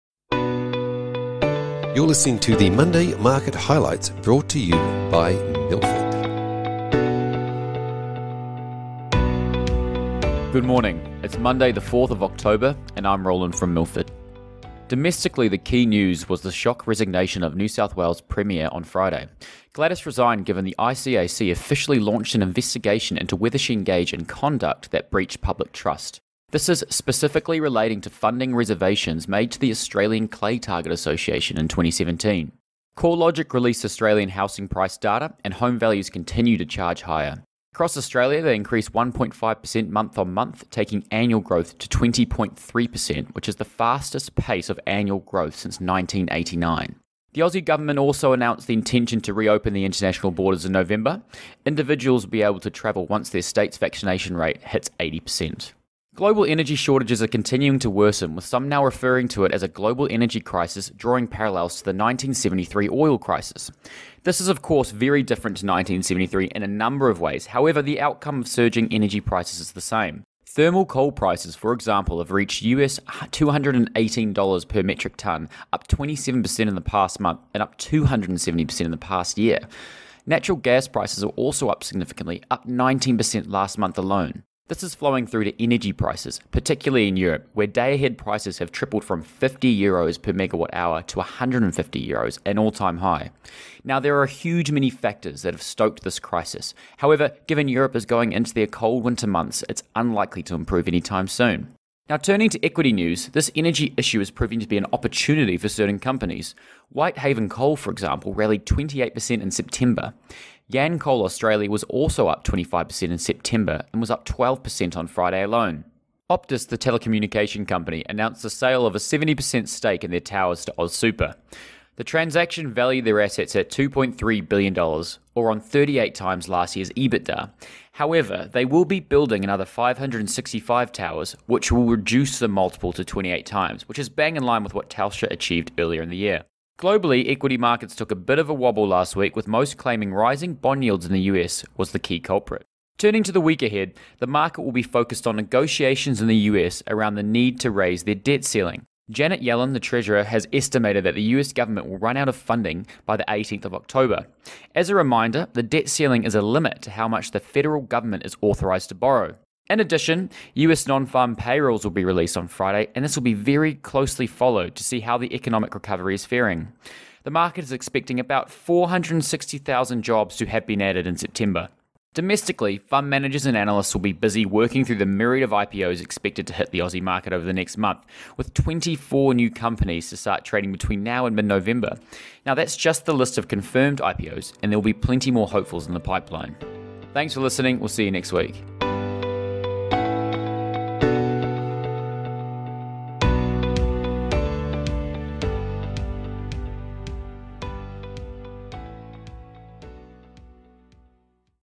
4 OCT 21 - Monday Market Highlights
Today we cover the shock resignation of NSW’s Premier, Global energy shortages, Optus, and the US debt ceiling.